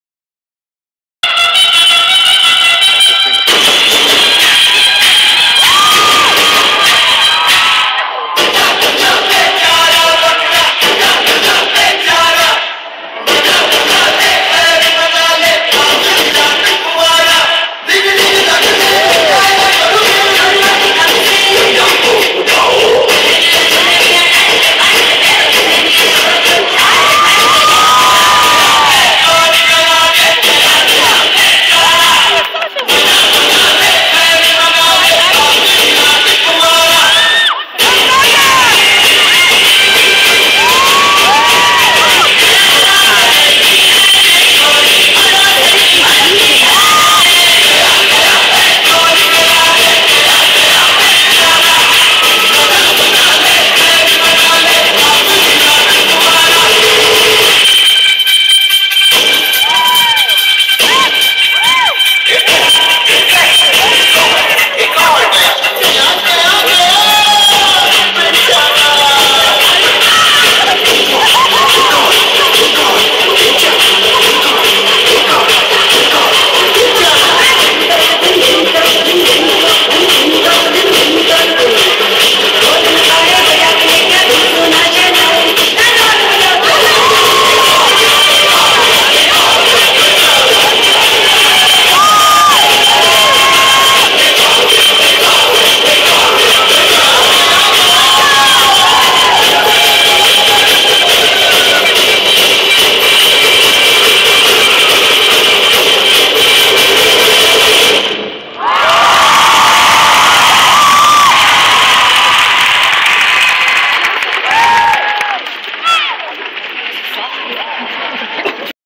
SYTYCD Tour 2011 Group Bollywood
Nassau Coliseum 10-3-11